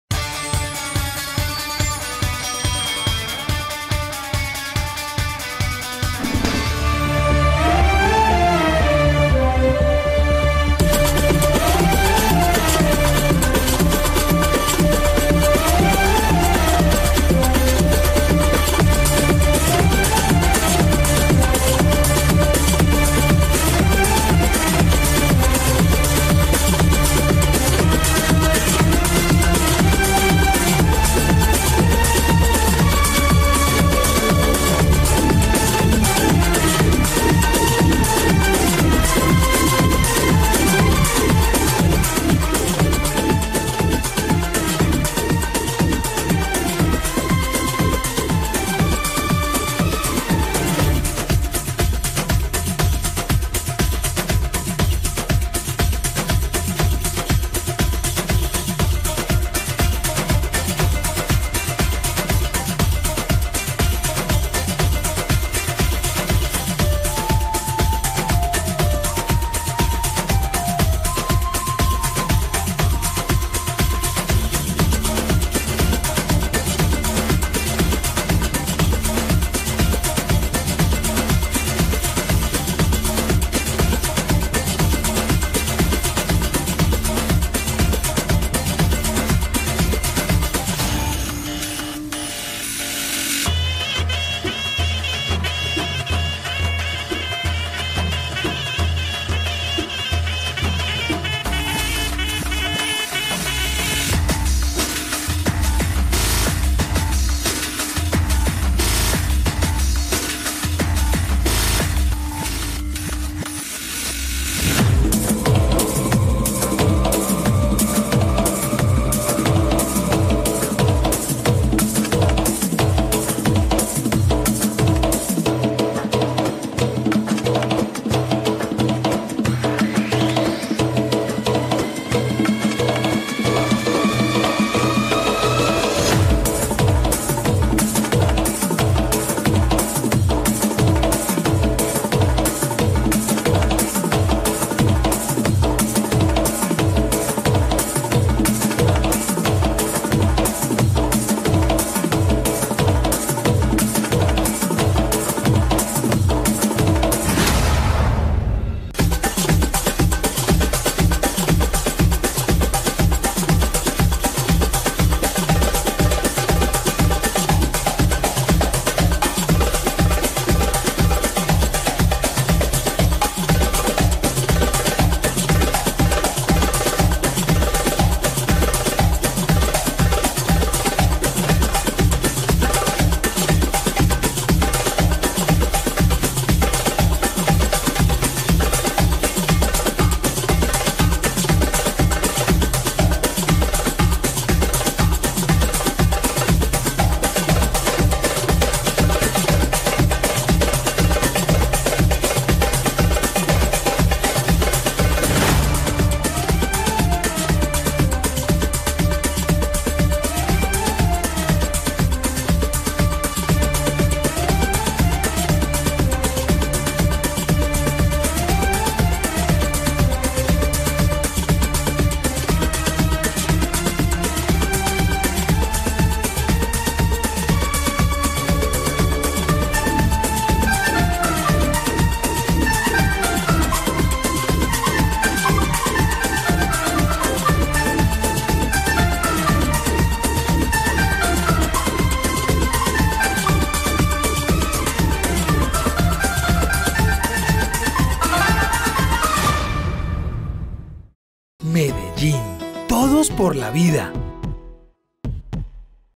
Reddo, categoría Danza: Comuna 14 - El Poblado
Los artistas de la Comuna 14 - El Poblado, hicieron gala de su talento en las Audiciones PPCultura